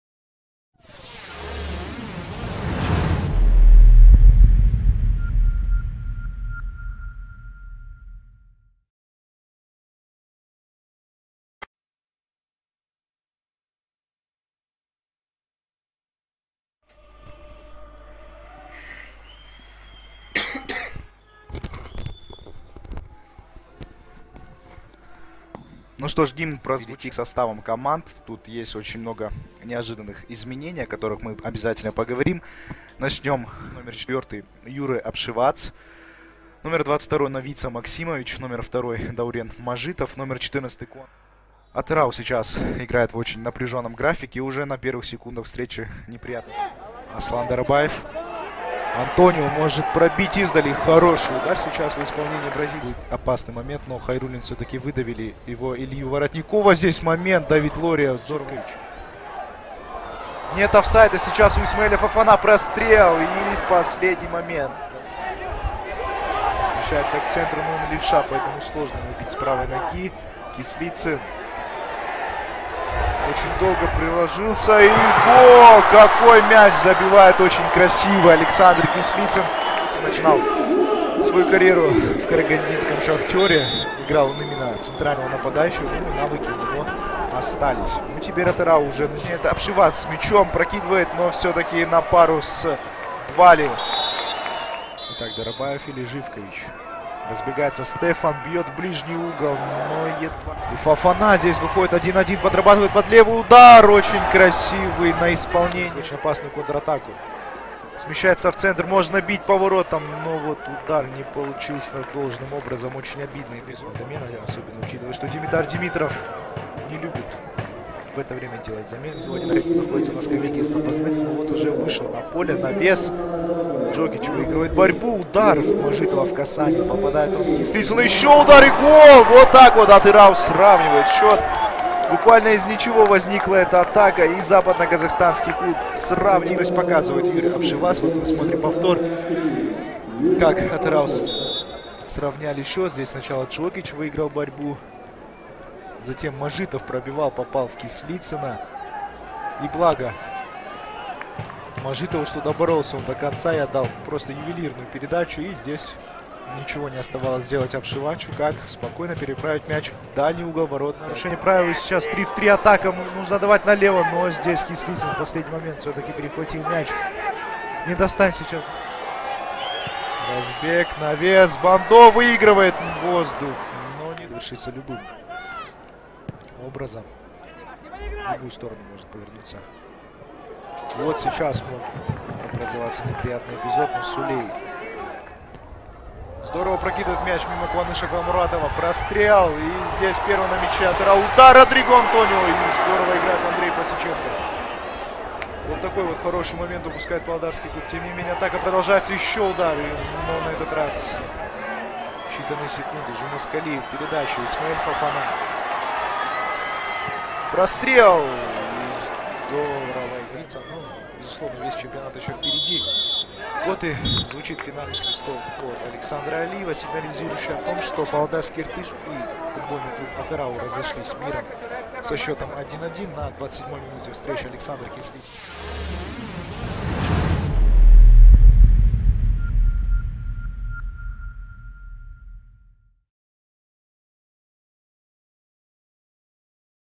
КПЛ-2017. 13 ТУР. ОБЗОР МАТЧА ИРТЫШ - АТЫРАУ 1:1
Иртыш - Атырау 1 :1 (1:0)

Голы: Кислицын Александр 27 (1:0) , Обшивач Юре 68 (1:1) .

Предупреждения: Родич Иван 44, Ли Виталий 69, Воротников Илья 72, Сапаров Алтынбек 80, Пасеченко Андрей 90.

Иртыш: Лория Давид (к), Фонсека Да Касто Фернандес Карлос Мануэл, Дарабаев Аслан (Алиев Пиралы 68), Воротников Илья, Гетериев Казбек (Жумаскалиев Нурбол 79), Кислицын Александр, Фофана Беко Исмаел, Джа Джедже Франк (Бандо Нганбе Серж 73), Двали Лаша, Родриго Антонио До Нассименто, Живкович Стефан.
Резерв: Калмыков Никита, Шабалин Павел, Есимов Руслан, Калинин Илья, Чернышов Владислав, Ткачук Евгений.

Атырау: Пасеченко Андрей, Хайруллин Марат (к) (Джокич Йован 46), Сапаров Алтынбек, Коробов Захар, Калмуратов Куаныш, Мажитов Даурен, Максимович Новица, Обшивач Юре, Родич Иван (Сулей Алишер 46 ), Лобжанидзе Уча, Двалишвили Владимир (Ли Виталий 65).
Резерв: Калменов Нурболат, Маров Алексей, Кайраллиев Даурен, Нурыбеков Айбар, Эсатов Руслан, Саломов Шавкат.

Судьи: Александр Алиев, Александр Павлов, Сергей Васютин (все -Алматы) . Резервный судья: Аскар Букенов (Павлодар) . Дополнительные ассистенты судей: Тимур Кумашев (Астана), Иван Филатов (Сарань).

20 мая, 2017, 19:00 . г.Павлодар, стадион "Центральный". 3500 зрителей.